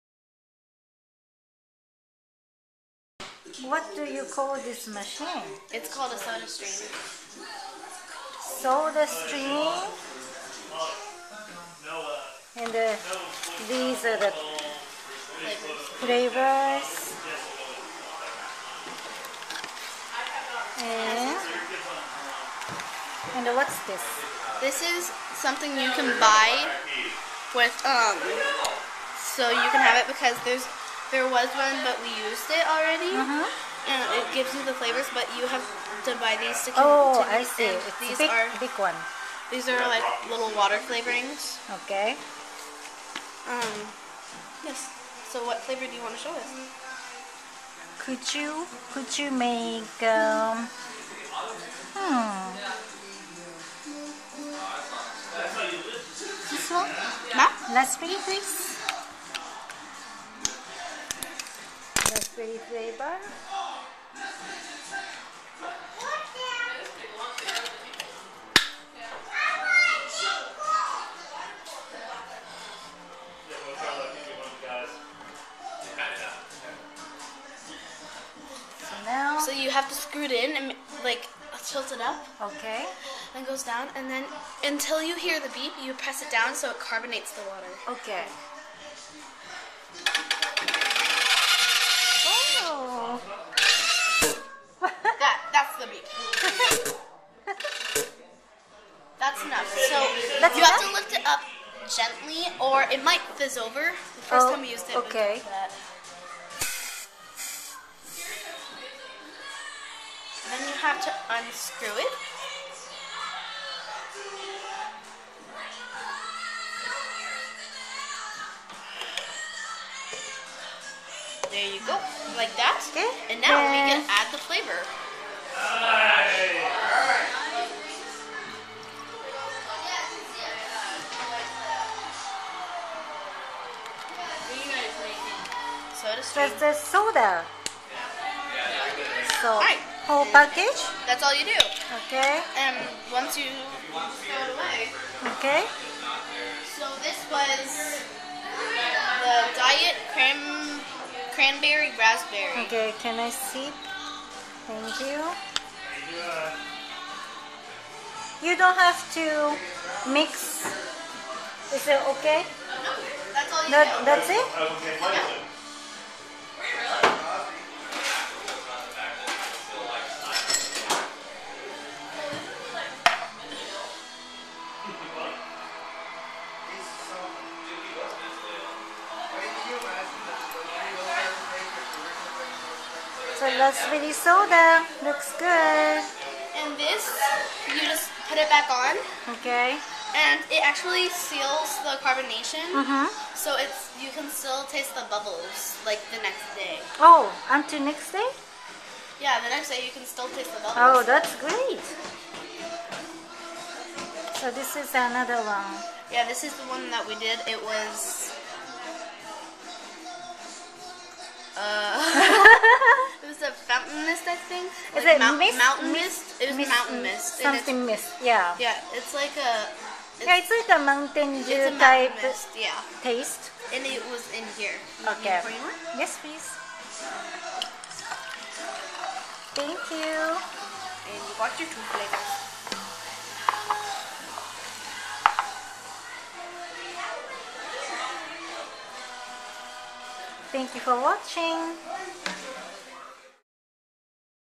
TOMOKO'S KITCHEN Soda Stream
I am introducing SodaStream.  Now you can make soda at home with this machine!  There is NO High Fructose Corn Syrup so it might be healther for you!  It also can make Tonic Water and Flavored Carbonated water.   Thank you for the demonstration, Allison.   Tomoko